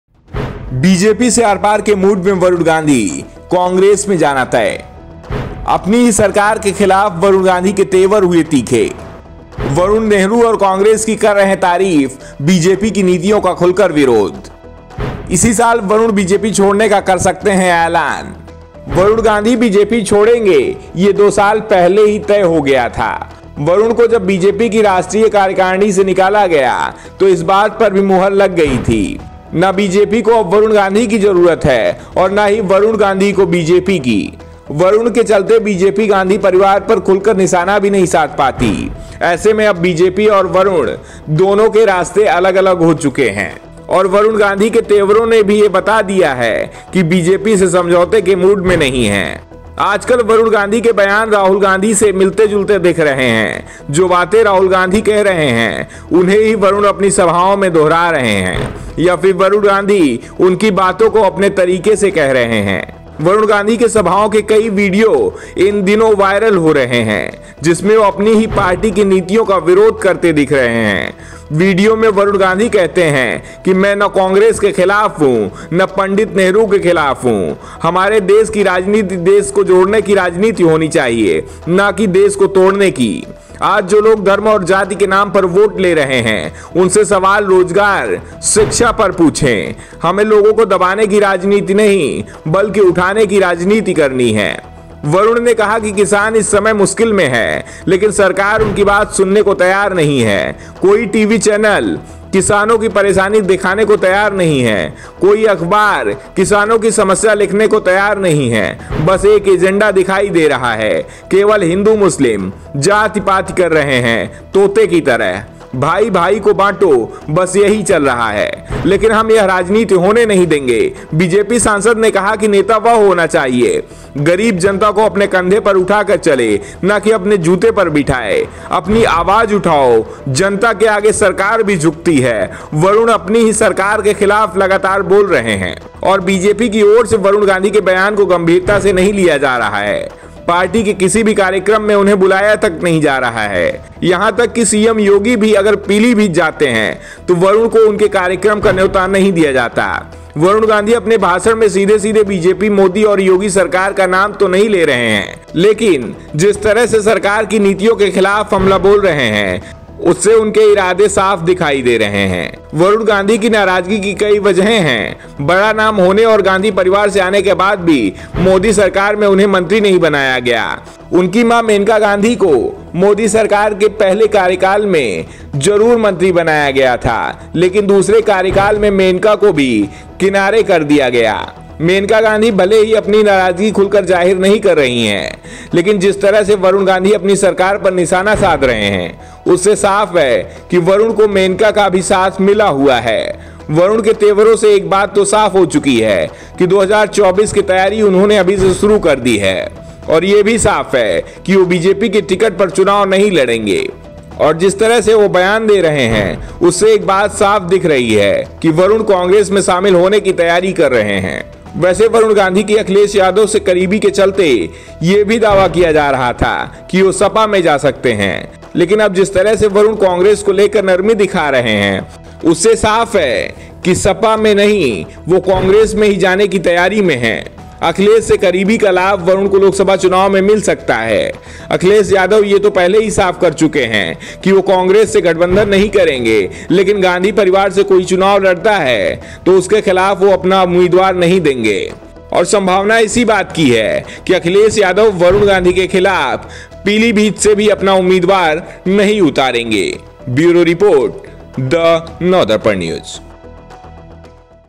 UP News : BJP से आर-पार के मूड में Varun Gandhi , Congress में जाना तय | THE NDN NEWS | Latest News
UP News : BJP से आर-पार के मूड में Varun Gandhi , Congress में जाना तय | THE NDN NEWS | Latest News | BJP Vs Congress |